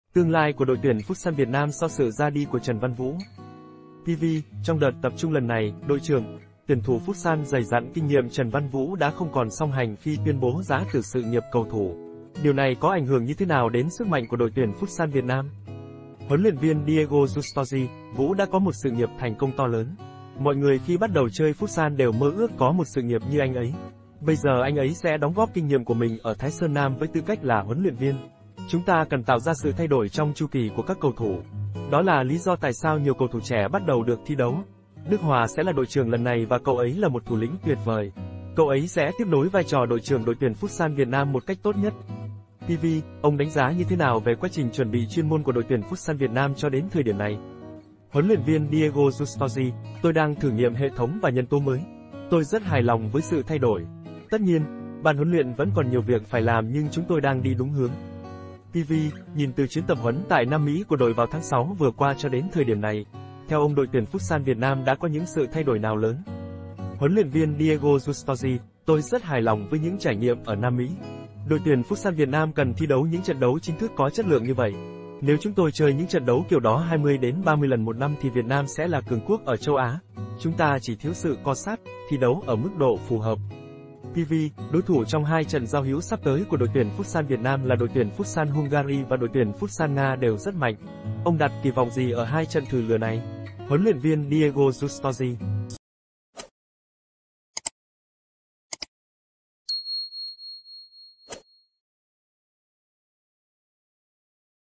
TƯƠNG LAI CỦA ĐỘI TUYỂN FUTSAL VIỆT NAM SAU SỰ RA ĐI CỦA TRẦN VĂN VŨ
PV: Trong đợt tập trung lần này, đội trưởng, tuyển thủ futsal dày dặn kinh nghiệm Trần Văn Vũ đã không còn song hành khi tuyên bố giã từ sự nghiệp cầu thủ. Điều này có ảnh hưởng như thế nào đến sức mạnh của  Đội tuyển futsal Việt Nam?